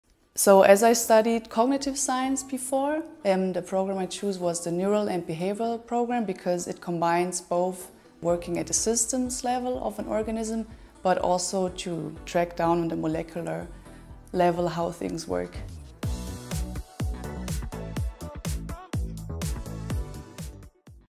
PhD student in Neuroscience 🧠 in Tübingen
Lílian is a PhD student in Neuroscience at the International Max Planck Research School IMPRS for the Mechanisms of Mental Function and Dysfunction MMFD. Read more about the 5-year Master of Science/PhD program